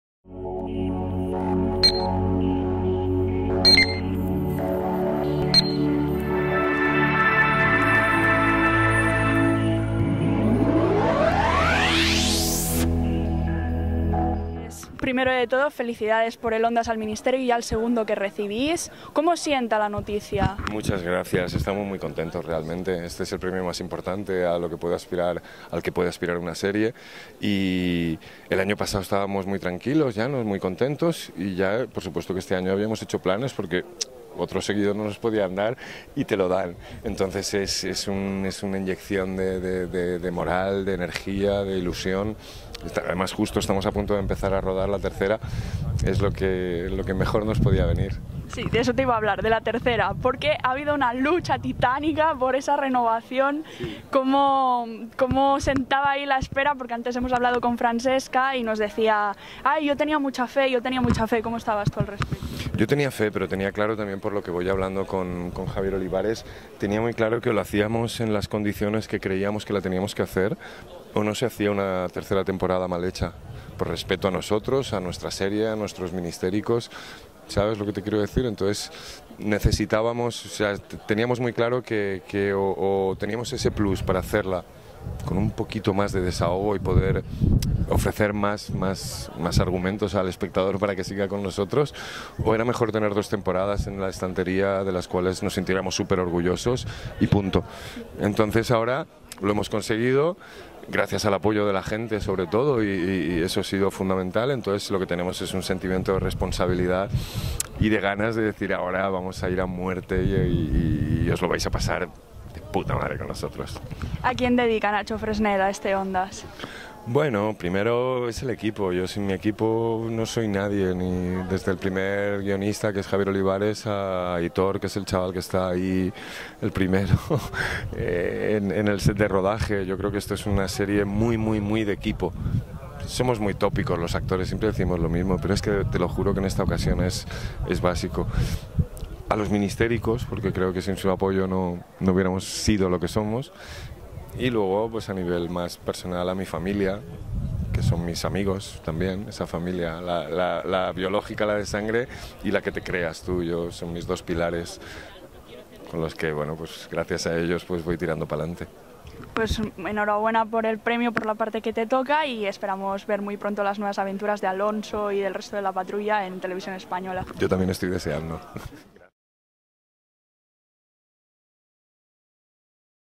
NACHO FRESNEDA "EL MINISTERIO DEL TIEMPO" | ENTREVISTA | PREMIOS ONDAS 2016 | NOVIEMBRE 2016
Entrevista: MARTA MOLINS
Cámara: SARA GIL